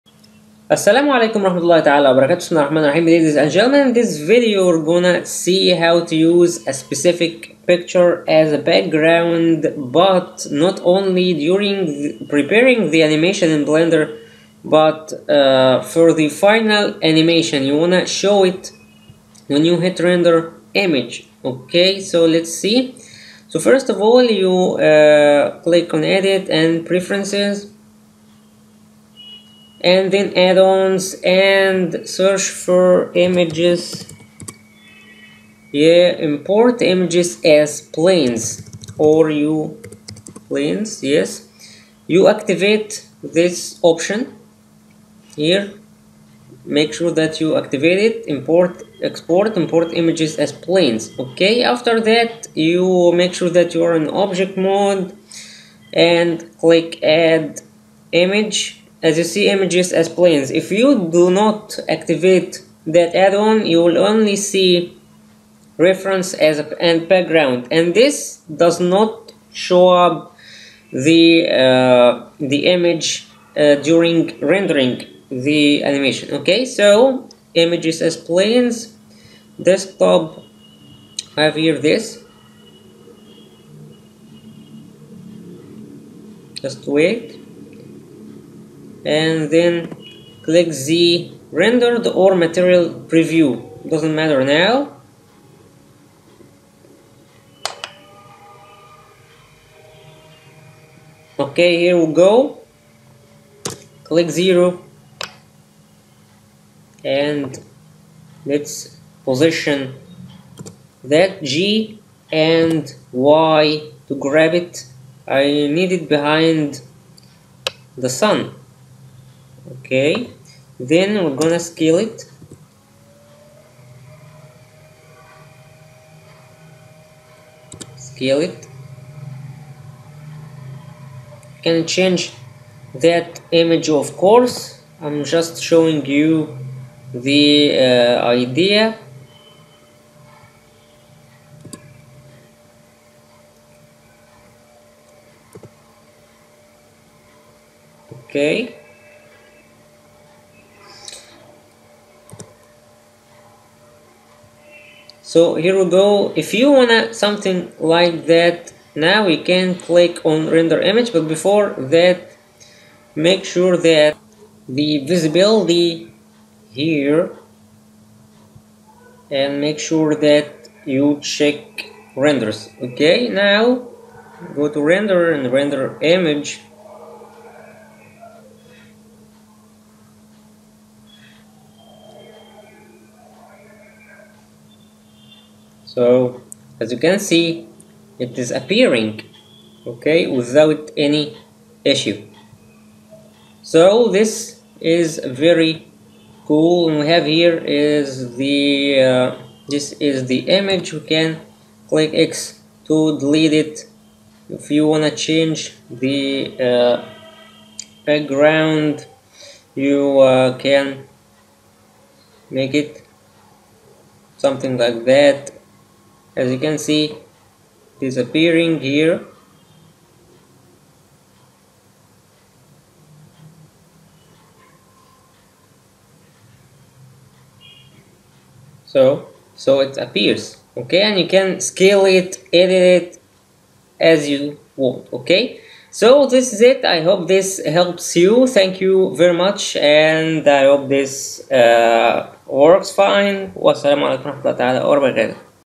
How to add an image as a background and make it visible in the rendered animation in blender 2.93
Adding a realistic scene to your animation's background is very cool. In blender 2.93, you can use the add-on import-export images as planes and make the image appear in the rendered animation.